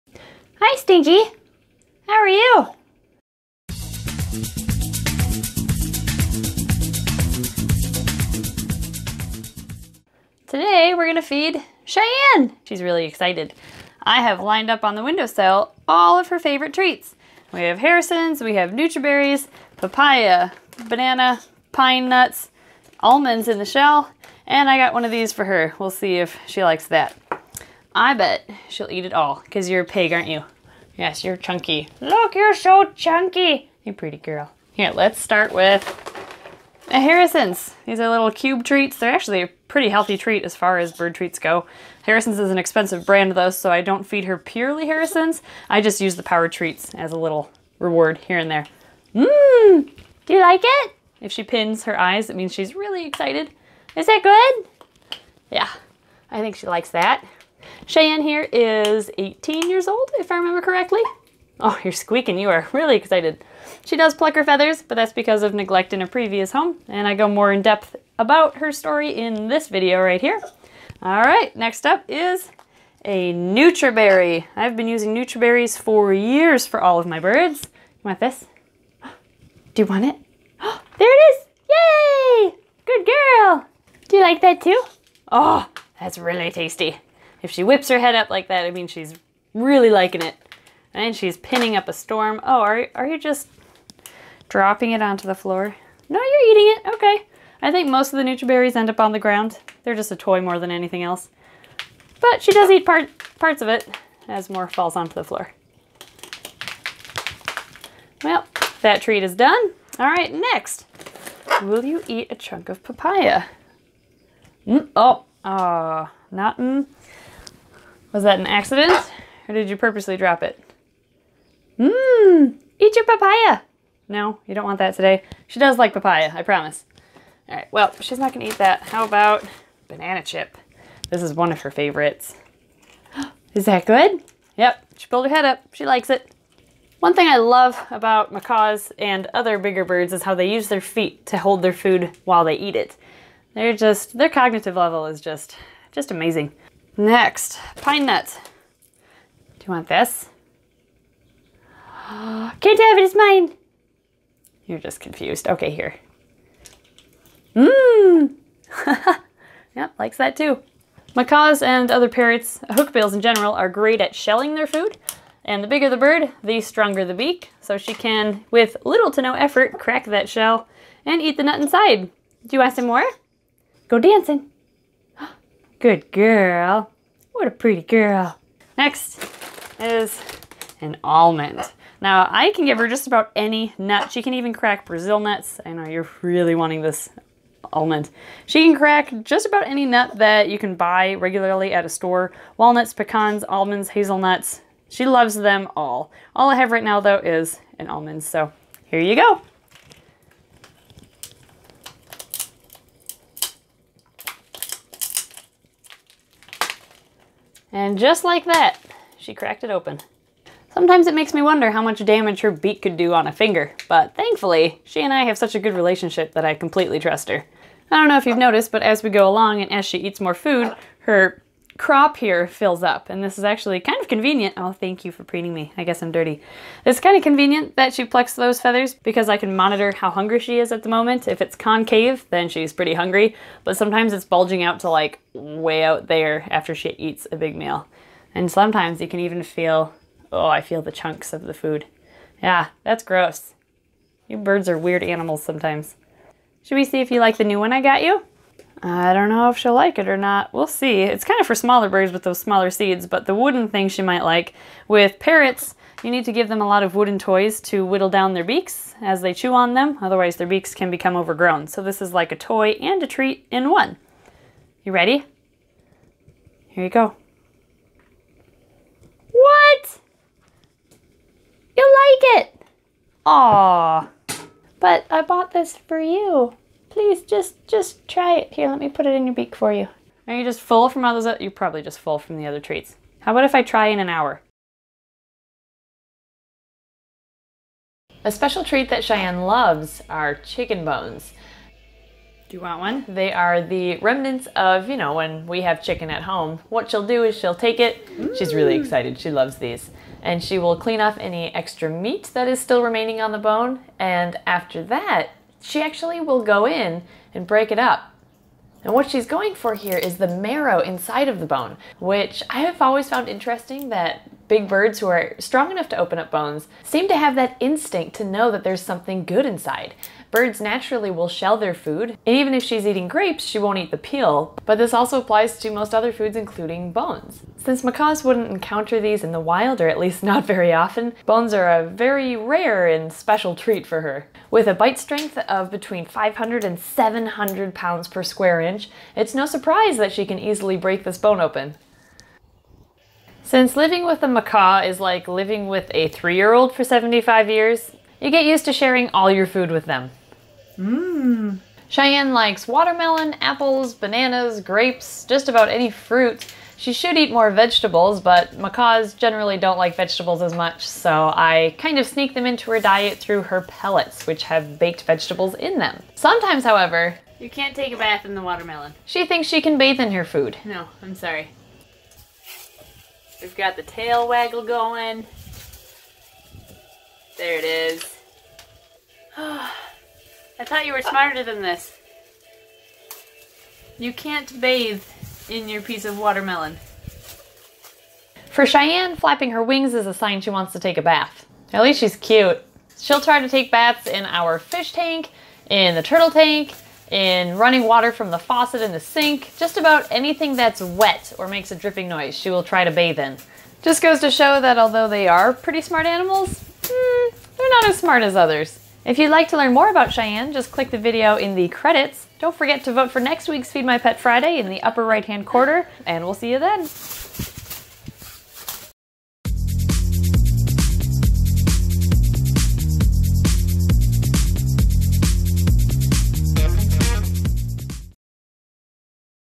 FMPF: My macaw is a little spoiled...
By popular demand, this week we feed my blue & gold macaw Cheyenne a variety of treats! And then she tries to bathe in one of them... (you'll see what I mean).

Note: If you're watching this and it's in poor resolution, I promise it will get better with time! Feel free to check back in a bit when YouTube is done processing. I would normally set it to "private" until it's done, but I'm exhausted so I'm going to bed instead :P